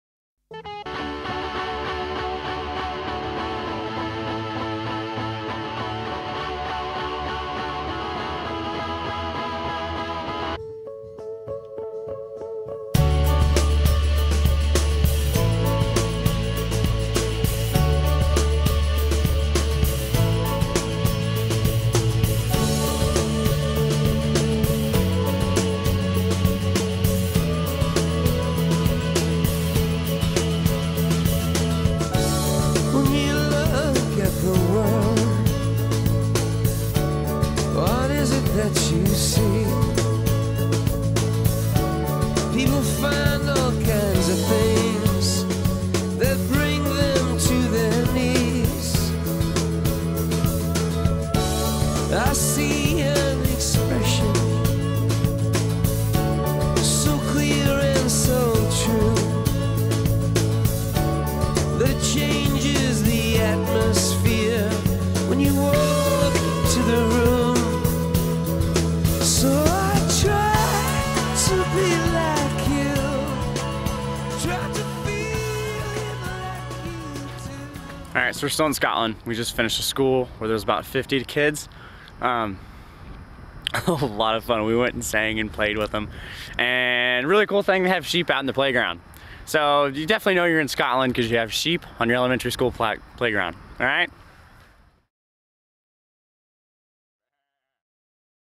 2004 ukscotland video journal | skene school
we stopped by a small school somewhere in Scotland. these kids were great.